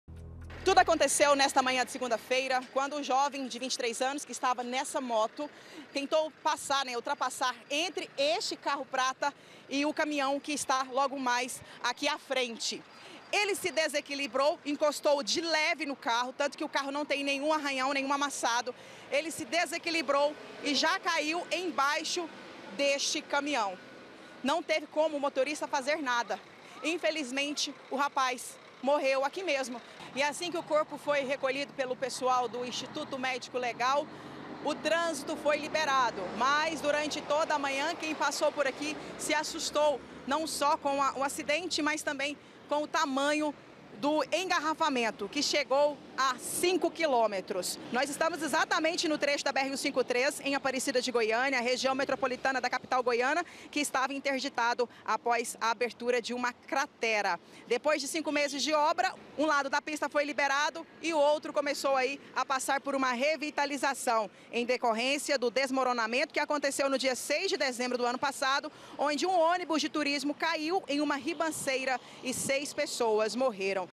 MORTE DESVIO BR 153
ACIDENTE GRAVE - MOTOCICLISTA MORRE EM TRECHO DE DESVIO DA BR 153
ACIDENTE GRAVE EM TRECHO DE DESVIO DA BR-153./ UM MOTOCICLISTA PERDEU O EQUILÍBRIO E ACABOU CAINDO EMBAIXO DE UM CAMINHÃO./